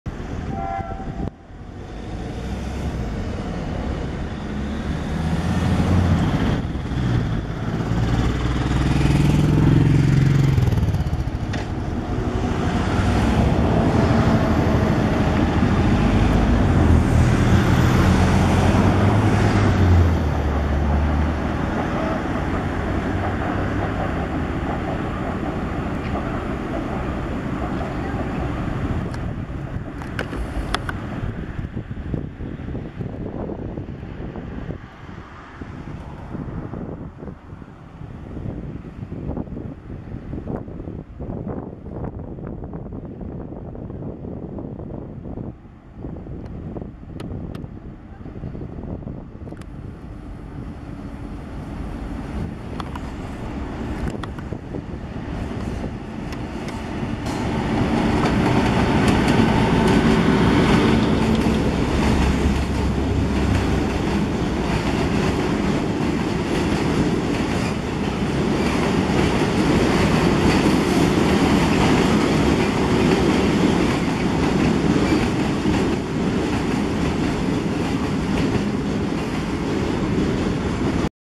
M62-0837 z kruszywem do Szopinka
Zamość, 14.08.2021 r. M62- 0837 ZiK Sandomierz z bocznicowym w Zamościu . W zasadzie to jeszcze nie bocznicowy, tylko zwykły pociąg ale nazwa historyczna, bo kiedyś były to pociągi bocznicowe, zanim zbudowano posterunek Szopinek. Jedzie kruszywo, które - co ciekawe było rozładowywane na terenie ciepłowni a nie byłej bocznicy PRD.